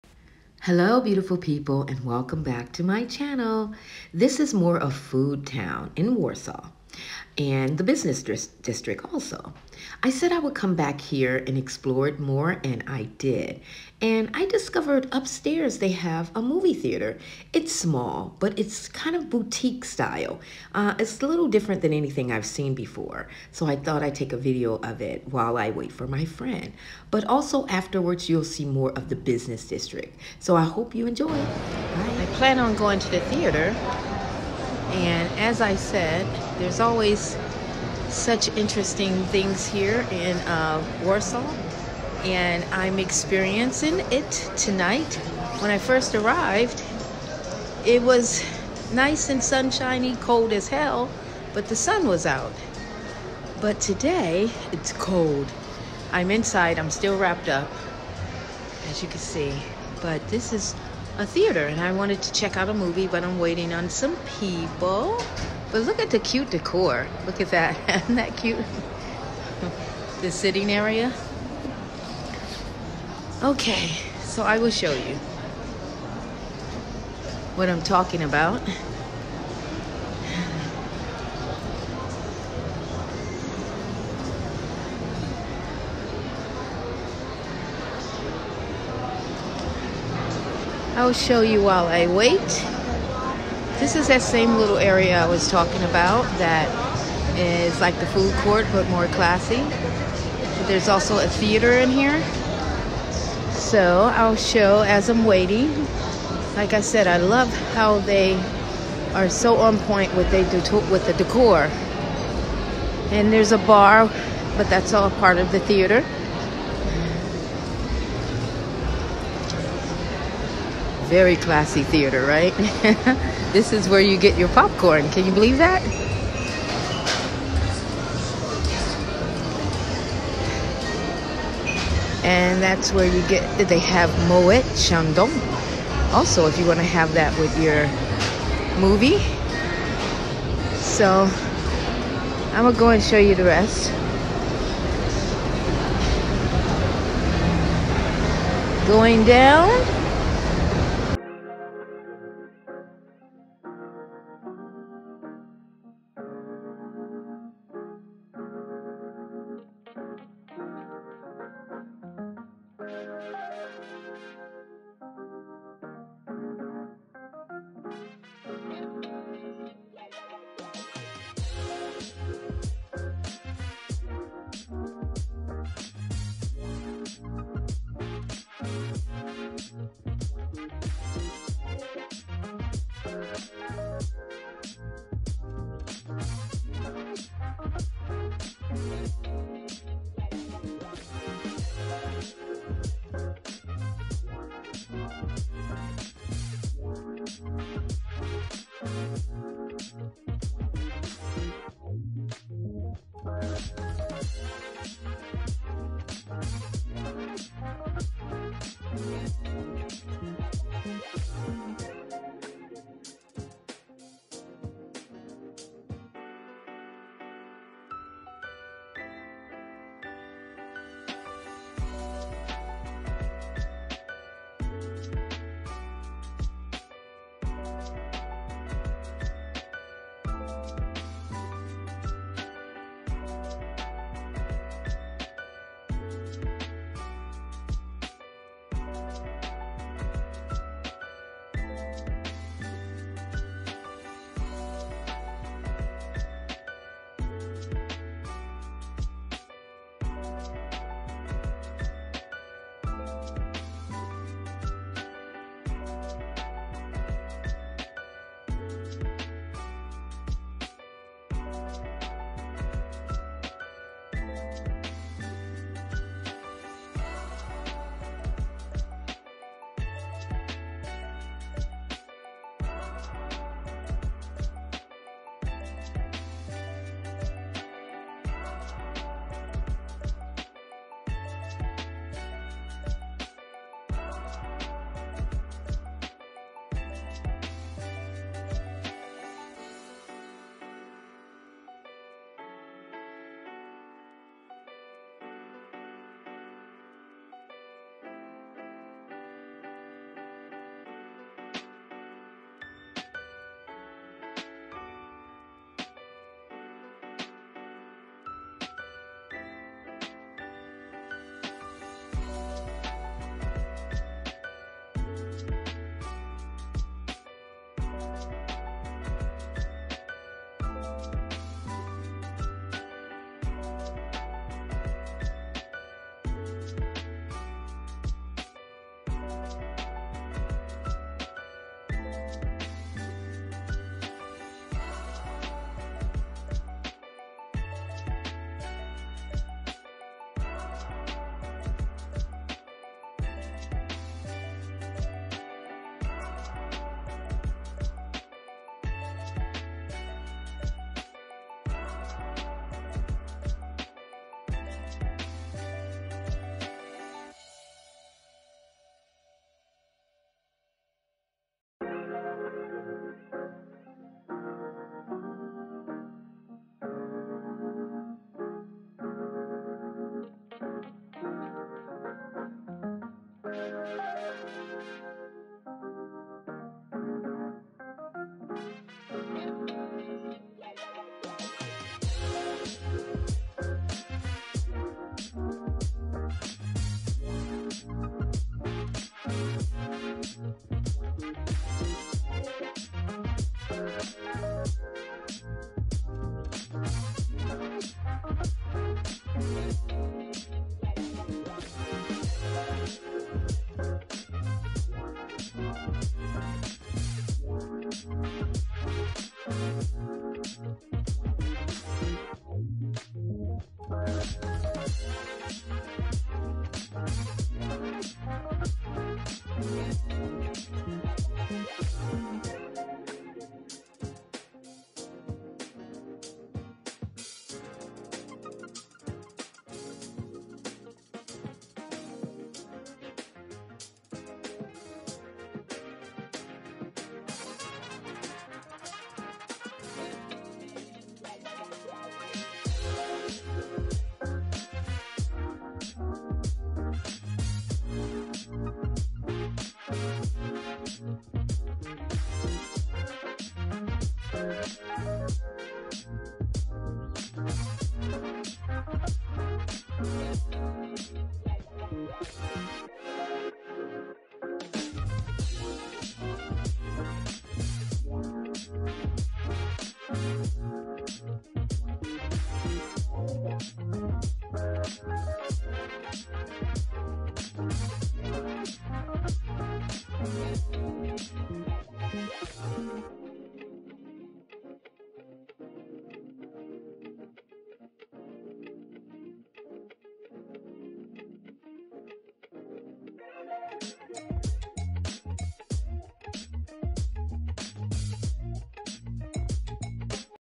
Poland keeps impressing me with some unique spots..🇵🇱💜
Why do I enjoy Warsaw Poland?!?! This is my second time coming back to this Food Town and I went to a Cinema inside and upstairs. Watch to see how Cool it is. There are not that many travelers like me who visit or even decide to stay in Poland longer than a few days and I do have my reasons why this is, but they miss out on so much that Warsaw has to offer other than the typical tourist spots. My goal is to showcase some of Warsaw's most unique and interesting places to Eat or hang out and check out.

linktr.ee/

Teena Williams